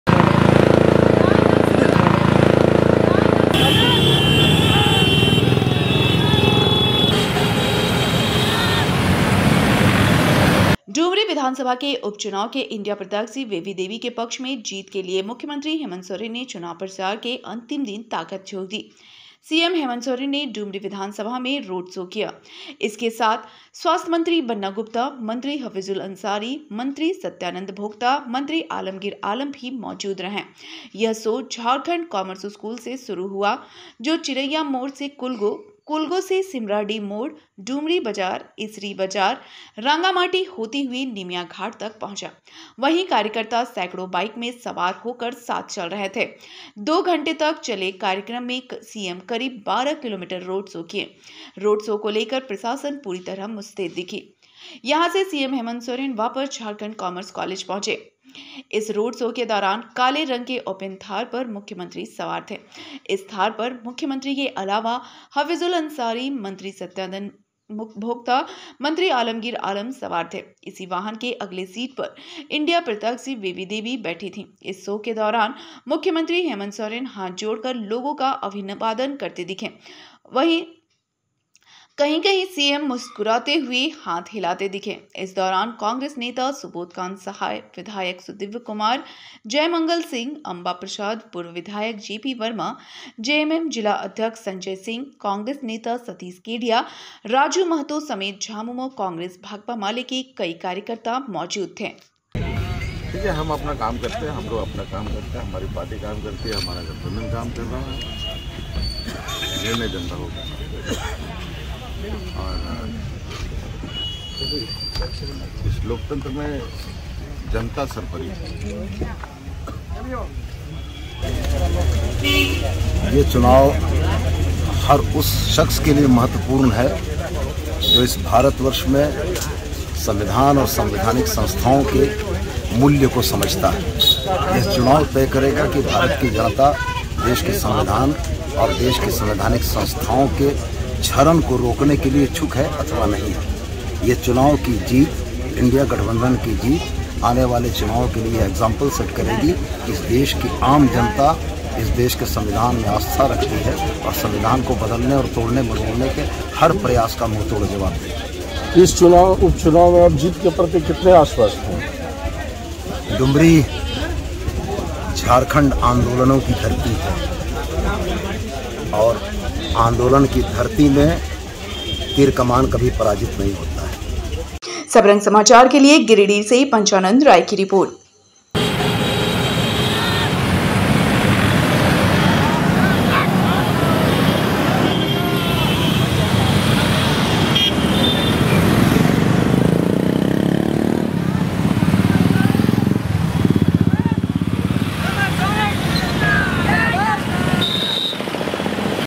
डुमरी उपचुनाव में सीएम हेमंत ने झोंकी ताकत, रोड शो में उमड़ा जनसैलाब...
डुमरी विधानसभा के उपचुनाव के इण्डिया प्रत्याशी बेबी देवी के पक्ष में जीत के लिए मुख्यमंत्री हेमंत सोरेन ने चुनाव प्रचार के अंतिम दिन ताकत झोंक दी. सीएम हेमंत सोरेन ने डुमरी विधानसभा में रोड शो किया. इनके साथ स्वास्थ्य मंत्री बन्ना गुप्ता,मंत्री हफीजुल अंसारी,मंत्री सत्यानंद भोक्ता,मंत्री आलमगीर आलम भी मौजूद रहे। यह शो झारखंड कॉमर्स कॉलेज से शुरू हुआ जो चिरैया मोड़ से कुलगो, कुलगो से सीमराडीह मोड़, डुमरी बाजार, इसरी बाजार,रांगामाटी होते हुए निमियाघाट तक पहुंचा.वहीं कार्यकर्ता सैकड़ो बाइक में सवार होकर साथ चल रहे थे। दो घंटे तक चले कार्यक्रम में सीएम करीब 12 किमी रोड शो किया। रोड शो को लेकर प्रसाशन पूरी तरह मुस्तैद दिखी।